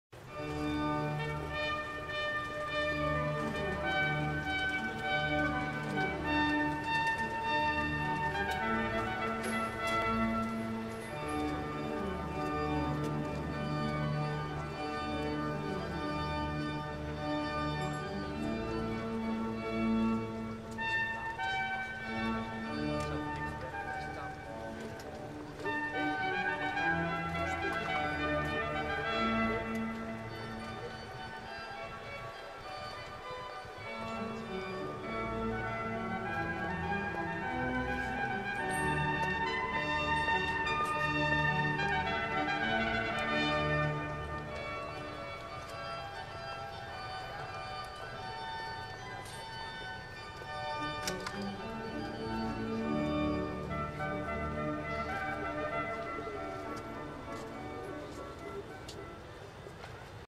2013 12 10 Eén minuut een lichtproject op de B Bijvoetbrug
B,Bijvoet wie kent hem niet? Vanaf zijn brug genoot ik van de openluchtkunst. Het is goed Amsterdammer te zijn.